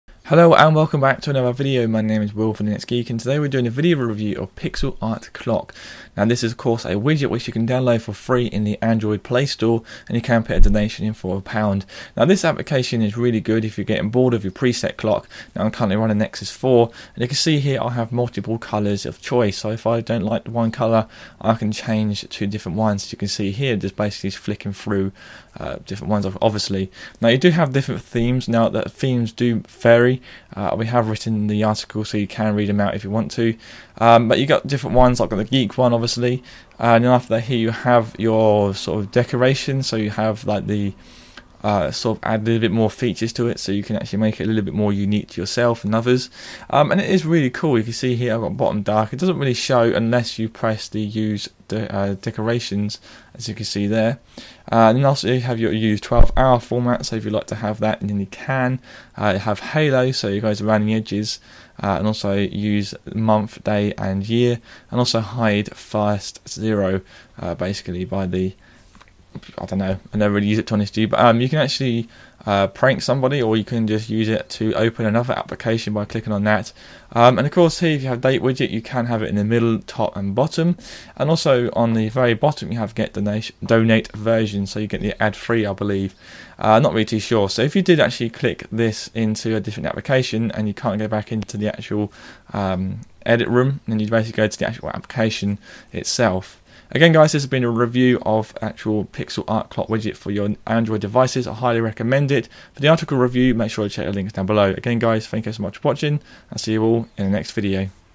Pixel Art Clock Widget: Review & Demo
In this video Will is reviewing this amazing Pixel Art Clock Widget for your Android devices. Will is currently using the Google Nexus 4 which he hasn't yet seen any issues as of yet. The widget is free to download but there is a paid version. For more information please visit the links bellow.